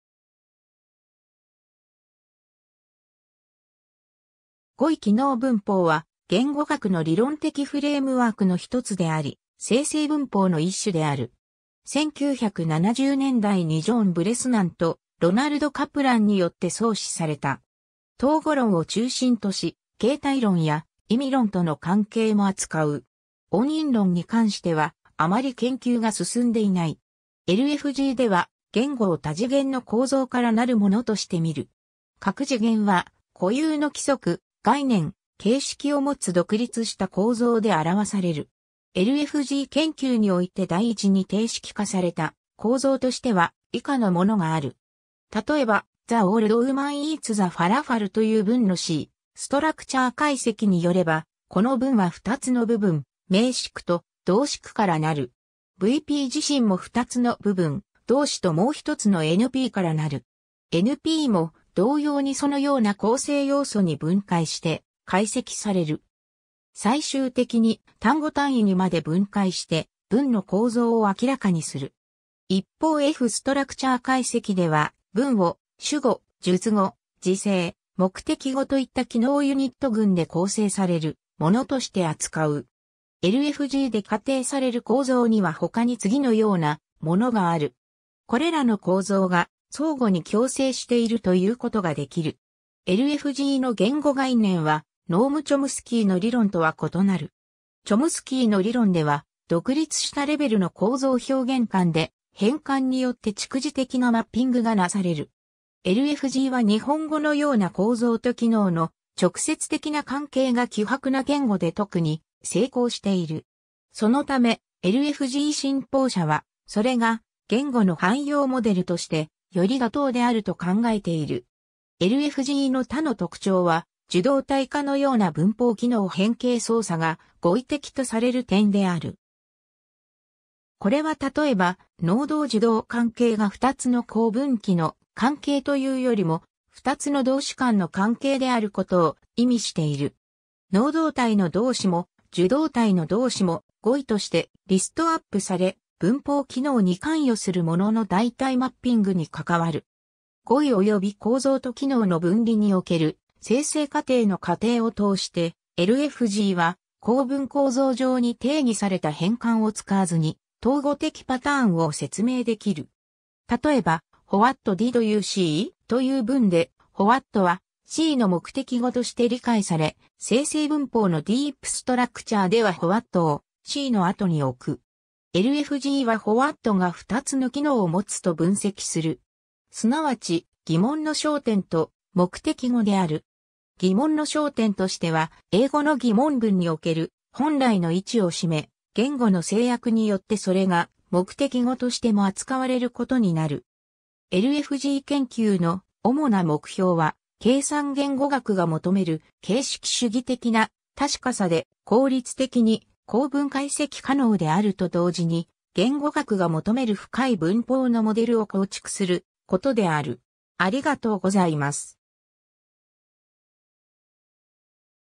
語彙機能文法
語彙機能文法（英: Lexical functional grammar, LFG）は、言語学の理論的フレームワークの1つであり、生成文法の一種である。
1970年代に Joan Bresnan と Ronald Kaplan によって創始された。
統語論を中心とし、形態論や意味論との関係も扱う。
音韻論に関しては、あまり研究が進んでいない（ただし、LFG の研究では最適性理論の考え方が一般化している）。
 LFG では、言語を多次元の構造からなるものとして見る。
各次元は、固有の規則・概念・形式をもつ独立した構造で表される。
LFG 研究において第一に定式化された構造としては、以下のものがある。
 例えば、The old woman eats the falafel（その老女はファラフェルを食べる）という文の c-structure 解析によれば、この文は2つの部分、名詞句 (NP) と動詞句 (VP) から成る。
VP 自身も2つの部分、動詞 (V) ともう1つの NP から成る。
NP も同様にそのような構成要素に分解して解析される。
最終的に、単語単位にまで分解して、文の構造を明らかにする。
一方 f-structure 解析では、文を主語、述語、時制、目的語といった機能ユニット群で構成されるものとして扱う。
 LFG で仮定される構造には他に次のようなものがある。
 これらの構造が「相互に強制している（mutually constraining）」と言うことができる。
 LFG の言語概念はノーム・チョムスキーの理論とは異なる。
チョムスキーの理論では、独立したレベルの構造表現間で、変換によって逐次的なマッピングがなされる。
LFG は日本語のような構造と機能の直接的な関係が希薄な言語（非階層型言語）で特に成功している。
そのため、LFG 信奉者は、それが言語の汎用モデルとしてより妥当であると考えている。
 LFG の他の特徴は、受動態化のような文法-機能変形操作が語彙的とされる点である。
これは例えば、能動-受動関係が2つの構文木の関係というよりも、2つの動詞間の関係であることを意味している。
能動態の動詞も受動態の動詞も、語彙としてリストアップされ、文法機能に関与するものの代替マッピングに関わる。
 語彙および構造と機能の分離における生成過程の仮定を通して、LFG は構文構造上に定義された変換を使わずに統語的パターンを説明できる。
例えば、What did you see?（あなたは何を見たか?）という文で、what は see の目的語として理解され、生成文法の "deep structure" では what を see の後（目的語の通常の位置）に置く。
LFG は what が2つの機能を持つと分析する。
すなわち、疑問の焦点（question-focus）と目的語である。
疑問の焦点としては、英語の疑問文における本来の位置を占め、言語の制約によってそれが目的語としても扱われることになる。
 LFG 研究の主な目標は、計算言語学が求める形式主義的な確かさで効率的に構文解析可能であると同時に、言語学が求める深い文法のモデルを構築することである。